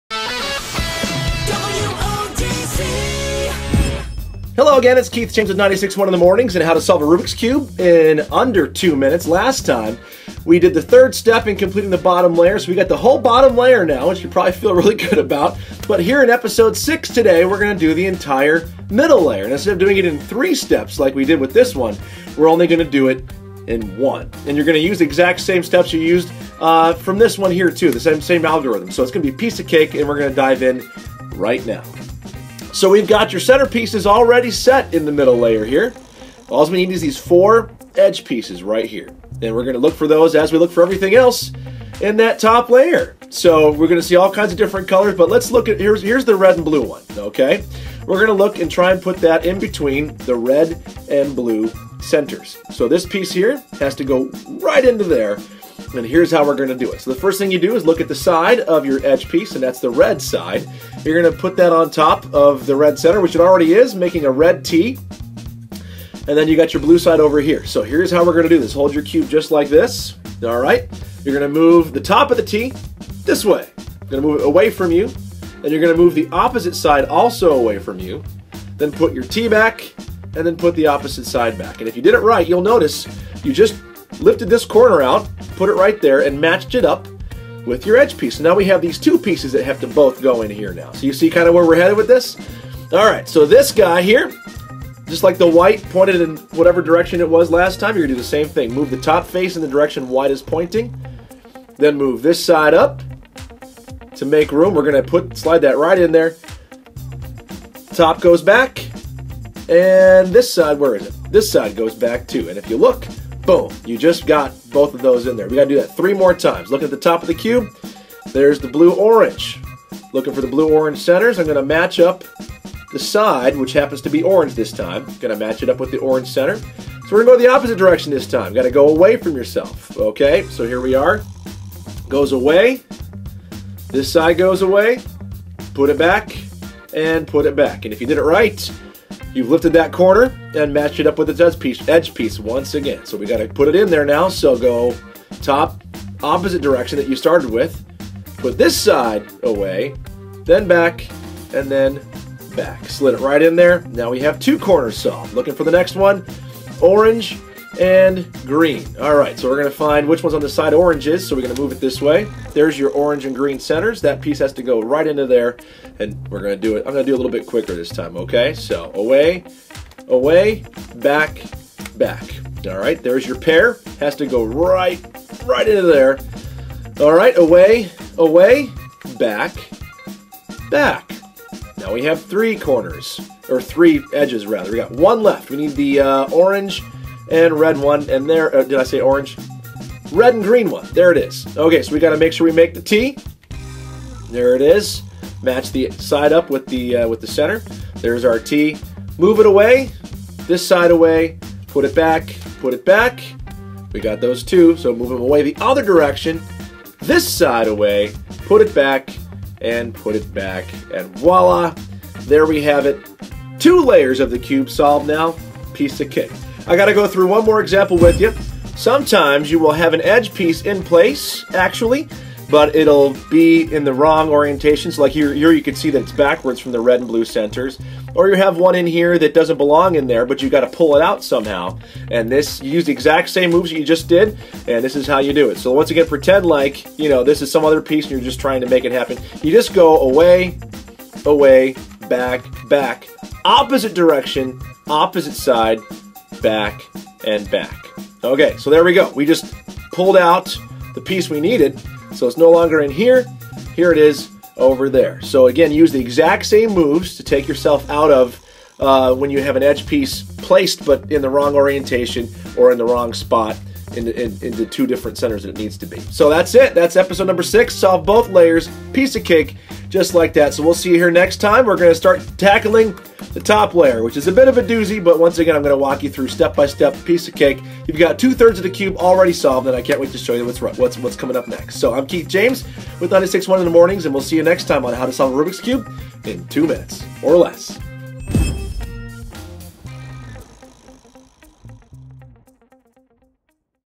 Crack The Cube w/ Keith James - The Middle Layer
EPISODE 6 - Now Keith James helps you tackle the middle layer, a little tougher but thankfully the steps we learned in the last episode will be used here and we only have four pieces to place too and we're done with 2/3rds of the Rubik's Cube!